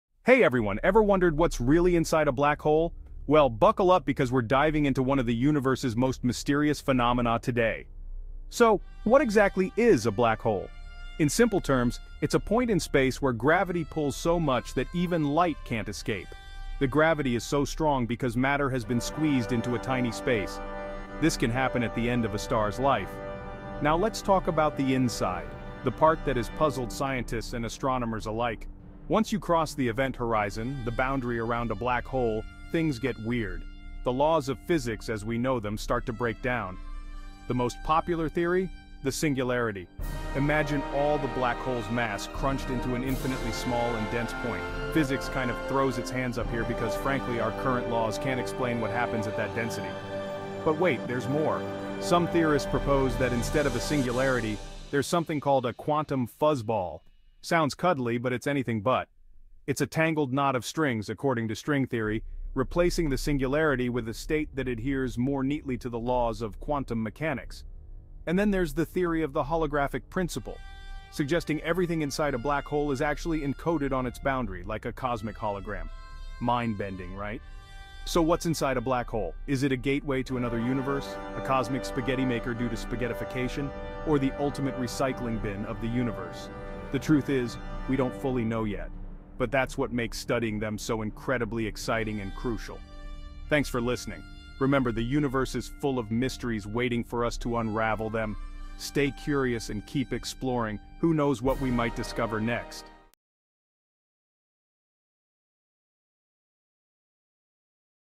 Journey to the Unknown Inside a Black Hole
Embark on a mind-bending journey into one of the universe's most mysterious phenomena: black holes. In this video, we explore the enigmatic nature of black holes, from their formation to the incredible gravitational forces at their core. Discover what theories suggest happens inside a black hole and the groundbreaking research that aims to unlock these cosmic secrets. Join us as we dive deep into the unknown and unravel the mysteries of these fascinating celestial objects.